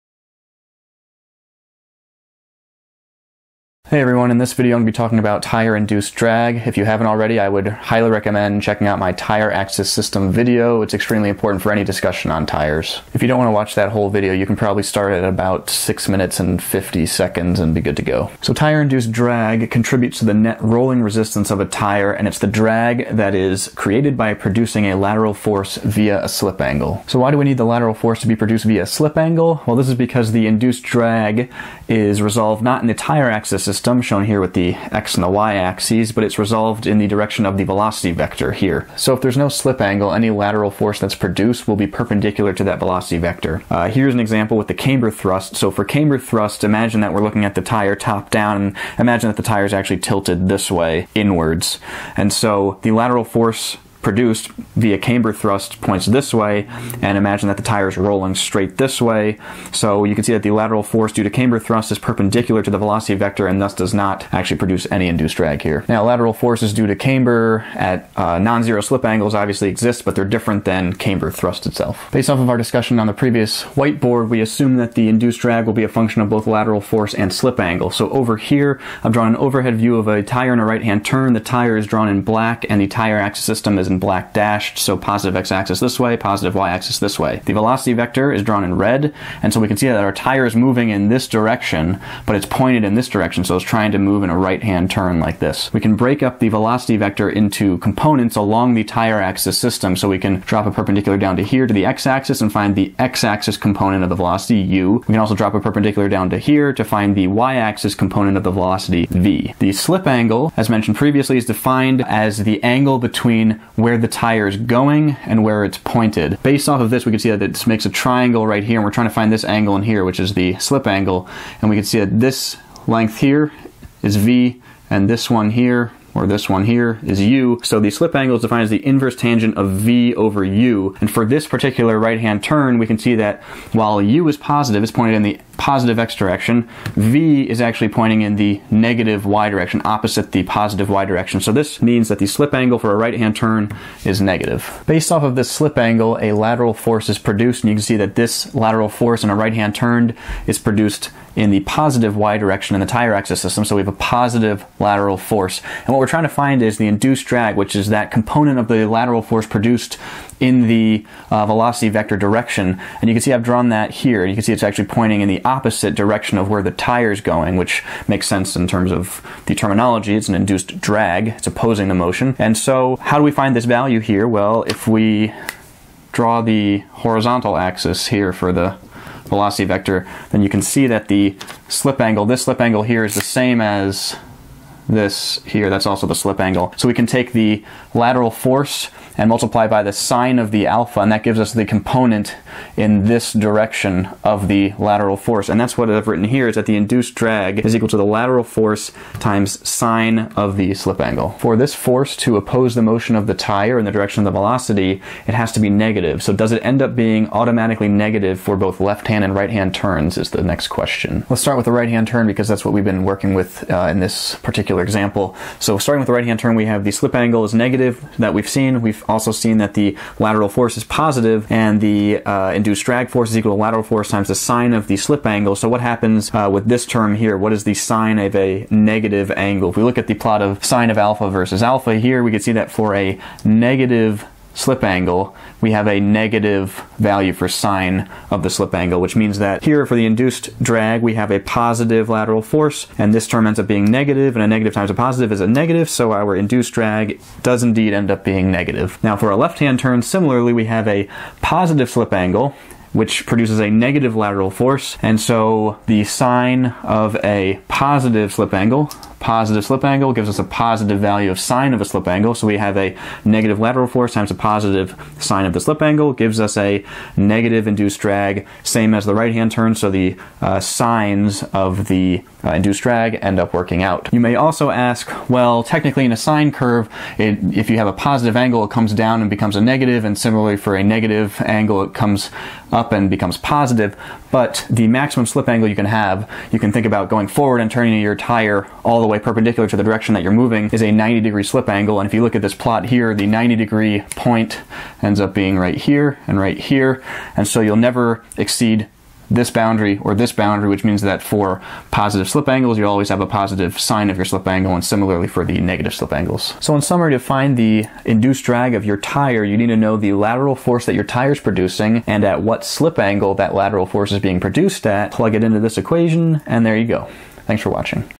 Explained: Tire Induced Drag
In this video we will define what tire induced drag is, and find an equation for it in terms of lateral force and slip angle.  The tire induced drag can be a significant portion of the rolling resistance acting on a car that's navigating a corner, such as on a racetrack.

For anyone worried/upset about my pronunciation of "camber" in this video, it was because I was working on some airfoil stuff before I filmed, and that's the way it's pronounced in aerodynamics.  I then made the executive decision to not film that section again because that would be a pain.

RELEVANT VIDEOS
→ Tire Axis System